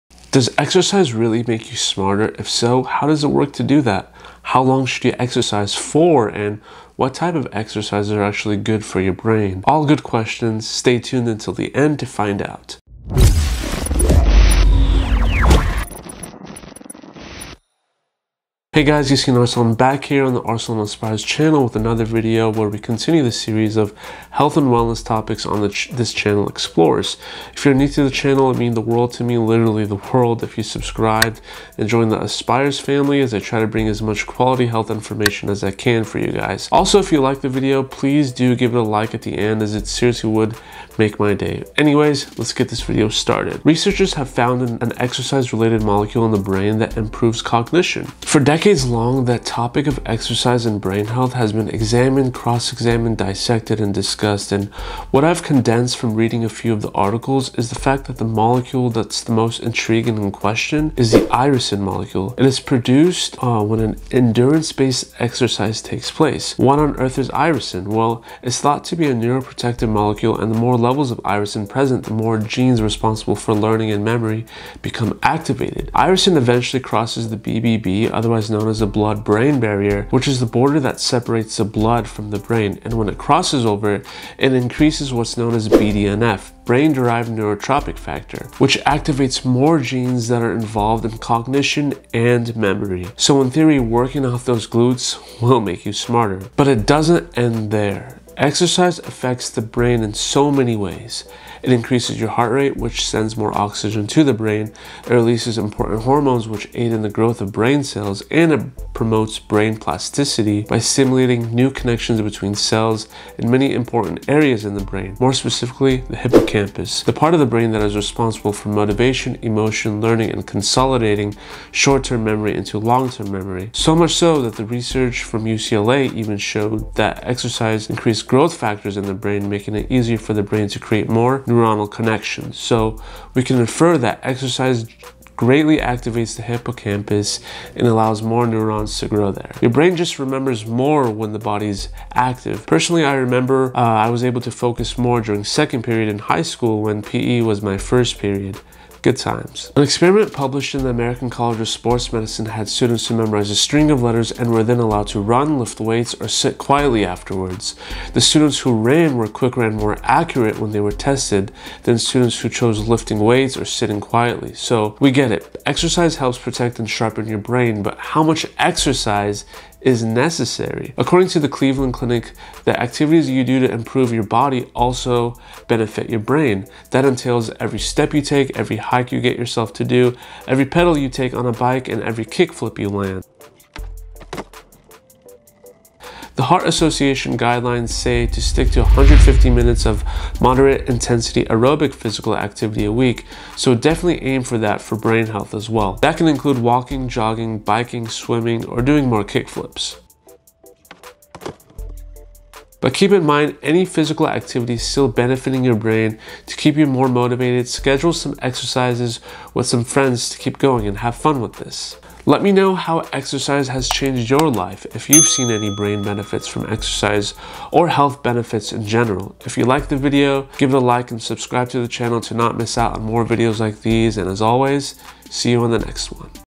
Does Exercise Really Make You Smarter?
These are the brain-changing benefits of exercise and how exercise benefits your brain! Find out how exercise is brain food!

Researchers have found an exercise related molecule in the brain that improves cognition. Irisin.

Irisin is produced in the brain when an endurance based exercise takes place. Its thought to be a neuroprotective molecule and the more levels of irisin present the more genes responsible for learning and memory become activated!

Irisin eventually crosses the BBB, the blood brain barrier which is the border that separates the blood from the brain, and when its crosses over, it increases what is known as BDNF (brain derived neurotrophic factor). BDNF activates more genes that are involved in cognition and memory.

But it doesn't end there, exercise affects the brain and the body in so many ways.

Exercise brain benefit #1
It increases your heart rate which sends more oxygen to the brain.

Exercise brain benefit #2
It releases important hormones which aid in the growth of brain cells.

Exercise brain benefit #3
It promotes brain plasticity by stimulating new connections between cells in many important areas in the brain.

More specifically, the hippocampus, the part of your brain that is responsible for motivation, emotion, learning, and consolidating short term memory into long term memory.